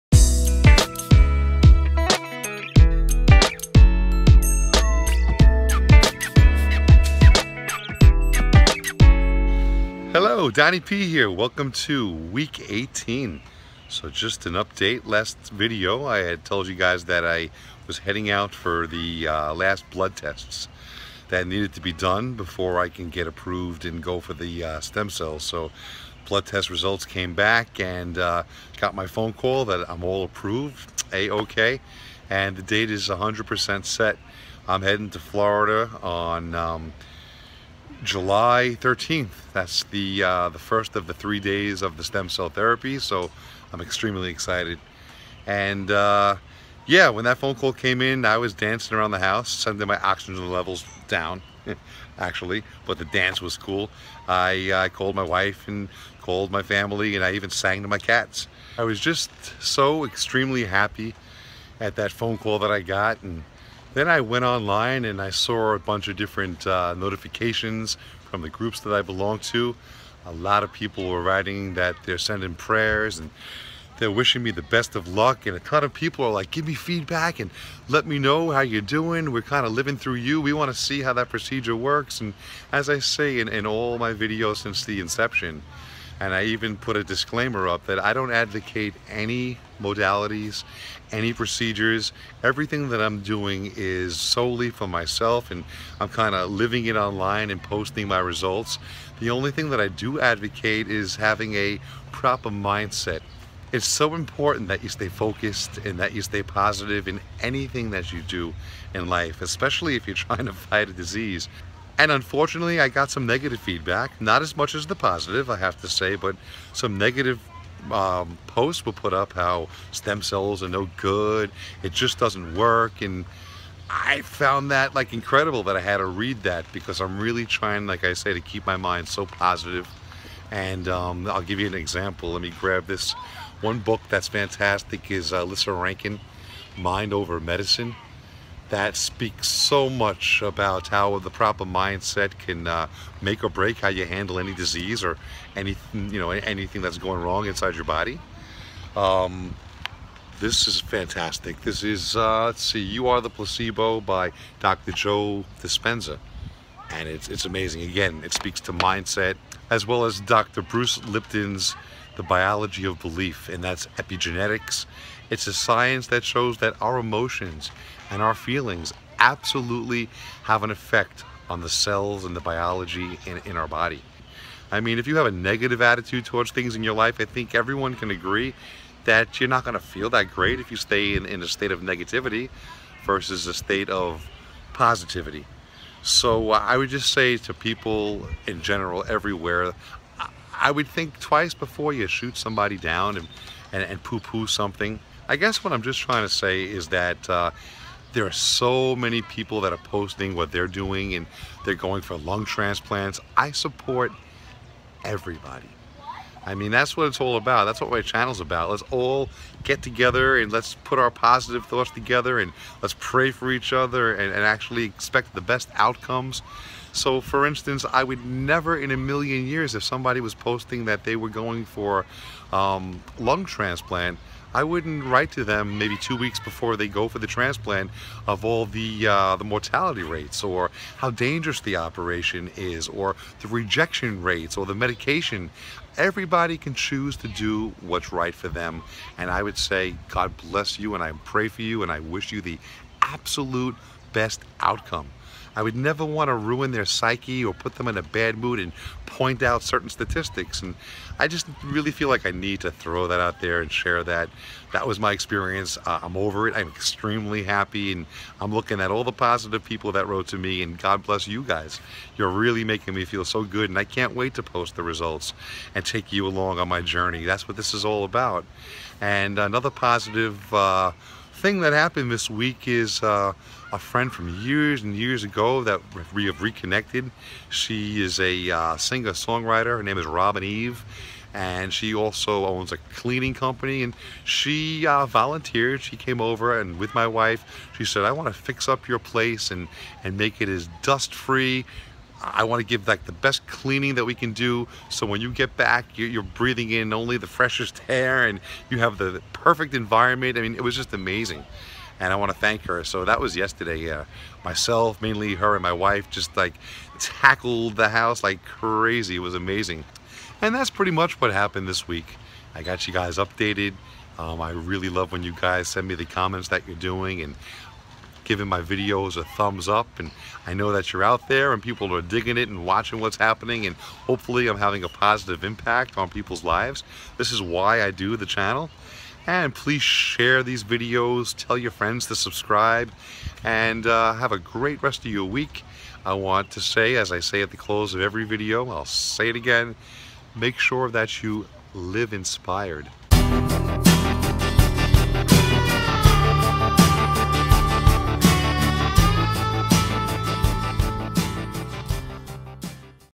Please Try To Stay Positive Don's Journey Beating PF #18
Blood Test Results Are Good!!!
Going for my Stem Cell Treatment on July 13th 14th & 15th!!!